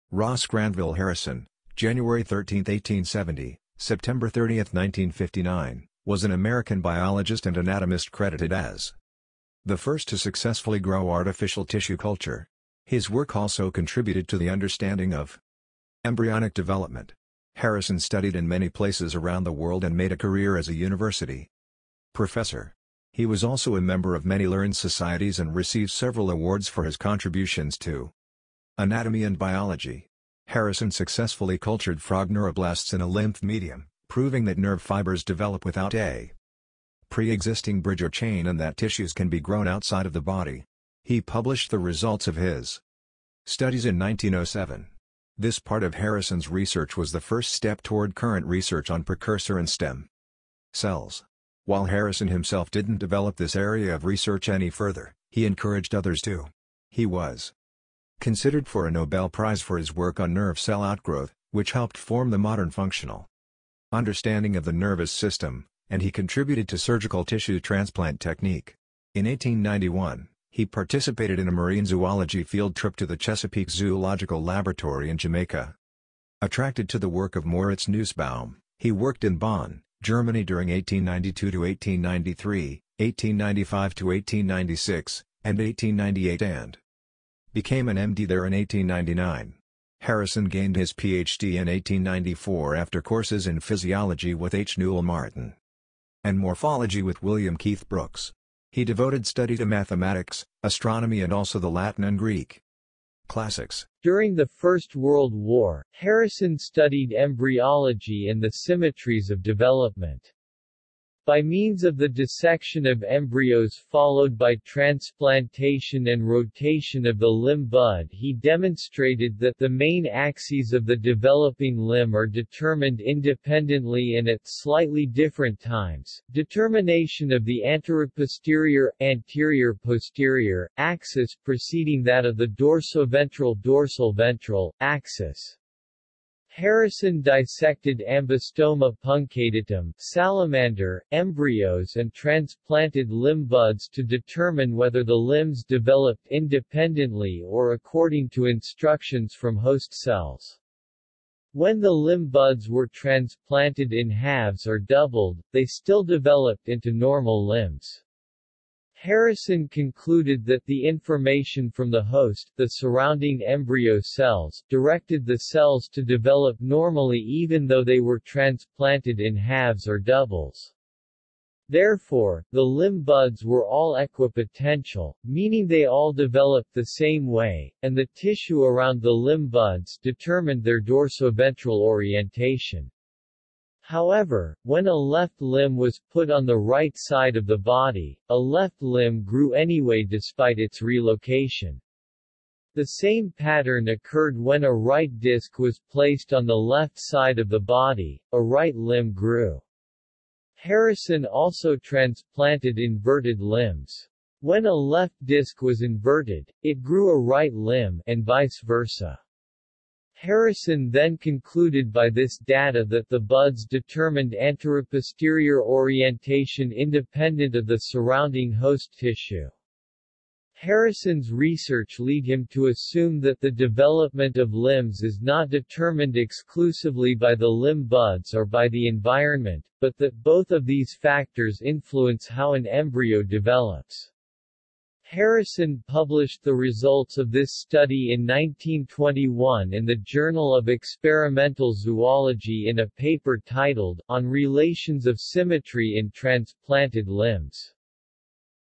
Tribute to Ross G Harrison an American biologist and anatomist who grew artificial tissue culture.
Ross Granville Harrison (January 13, 1870 – September 30, 1959) was an American biologist and anatomist credited as the first to successfully grow artificial tissue culture. His work also contributed to the understanding of embryonic development. Harrison studied in many places around the world and made a career as a university professor. He was also a member of many learned societies and received several awards for his contributions to anatomy and biology.

Harrison successfully cultured frog neuroblasts in a lymph medium, proving that nerve fibers develop without a preexisting bridge or chain and that tissues can be grown outside of the body. He published the results of his studies in 1907. This part of Harrison's research was the first step toward current research on precursor and stem cells. While Harrison himself didn't develop this area of research any further, he encouraged others to. He was considered for a Nobel prize for his work on nerve-cell outgrowth, which helped form the modern functional understanding of the nervous system, and he contributed to surgical tissue transplant technique.

In 1891, he participated in a marine zoology field trip to the Chesapeake Zoological Laboratory in Jamaica. Attracted to the work of Moritz Nussbaum, he worked in Bonn, Germany during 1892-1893, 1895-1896, and 1898 and became an M.D. there in 1899. Harrison gained his Ph.D. in 1894 after courses in physiology with H. Newell Martin and morphology with William Keith Brooks. He devoted study to mathematics, astronomy and also the Latin and Greek classics.

During the first world war, Harrison studied embryology and the symmetries of development. By means of the dissection of embryos followed by transplantation and rotation of the limb bud he demonstrated that the main axes of the developing limb are determined independently and at slightly different times, determination of the anteroposterior (anterior-posterior) axis preceding that of the dorsoventral (dorsal-ventral) axis. Harrison dissected Ambystoma puncatatum (salamander) embryos and transplanted limb buds to determine whether the limbs developed independently or according to instructions from host cells. When the limb buds were transplanted in halves or doubled, they still developed into normal limbs. Harrison concluded that the information from the host (the surrounding embryo cells) directed the cells to develop normally even though they were transplanted in halves or doubles. Therefore, the limb buds were all equipotential, meaning they all developed the same way, and the tissue around the limb buds determined their dorsoventral orientation. However, when a left limb was put on the right side of the body, a left limb grew anyway despite its relocation. The same pattern occurred when a right disk was placed on the left side of the body—a right limb grew. Harrison also transplanted inverted limbs. When a left disk was inverted, it grew a right limb (and vice versa). Harrison then concluded by this data that the buds determined anteroposterior orientation independent of the surrounding host tissue. Harrison's research lead him to assume that the development of limbs is not determined exclusively by the limb buds or by the environment, but that both of these factors influence how an embryo develops. Harrison published the results of this study in 1921 in the Journal of Experimental Zoology in a paper titled "On relations of symmetry in transplanted limbs".